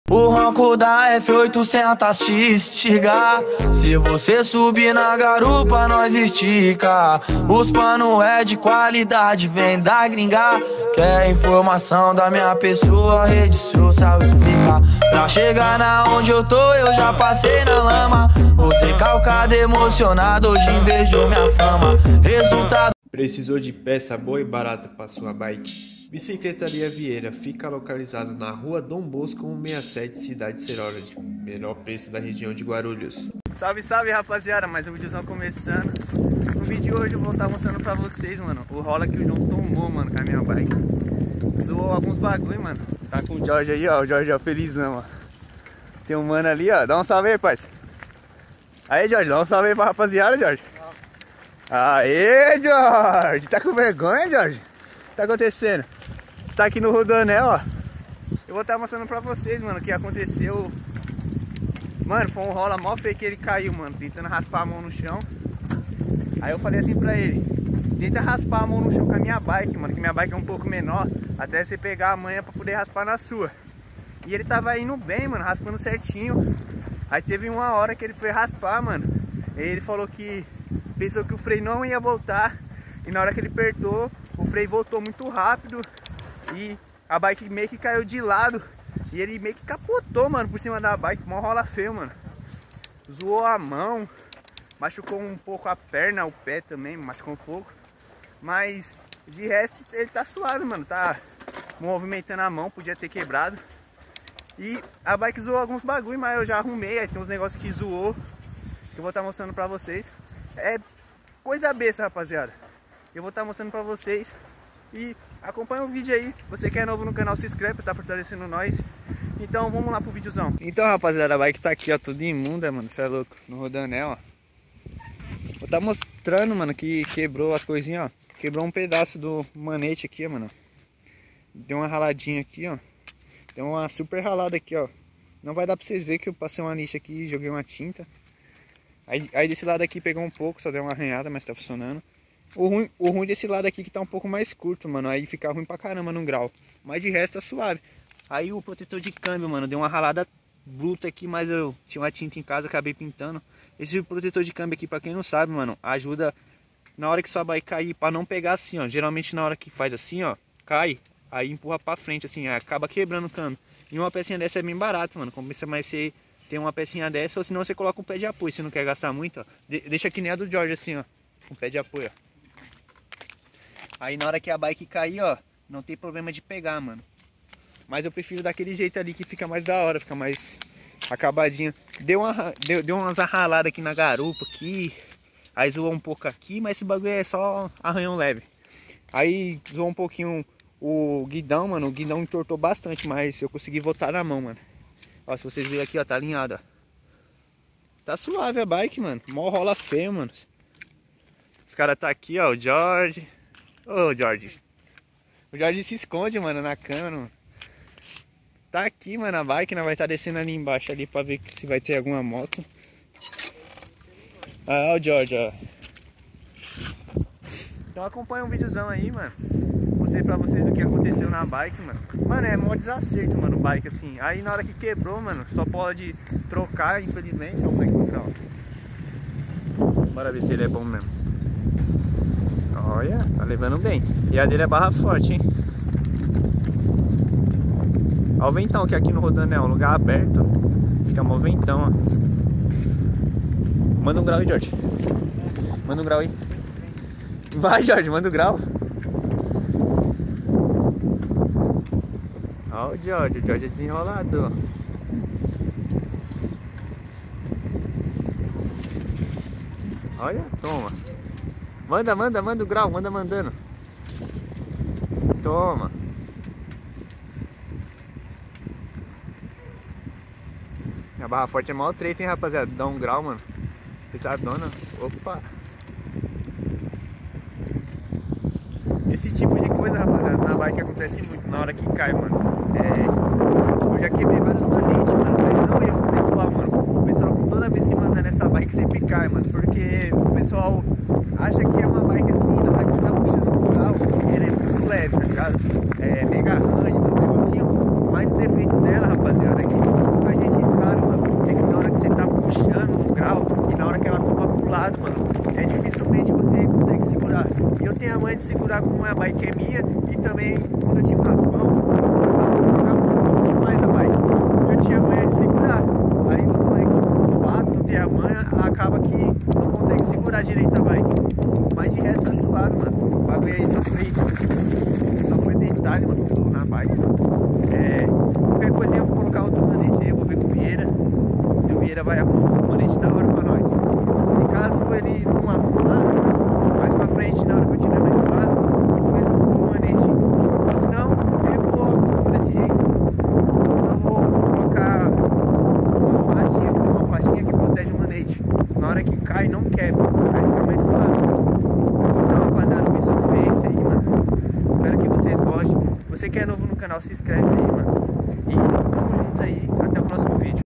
CAIU COM A MINHA BIKE
caiu com a minha bike quebrou alguma peça arranhou outras mais o mais importante que o mano que caiu nao se machucou tanto poderia ser pior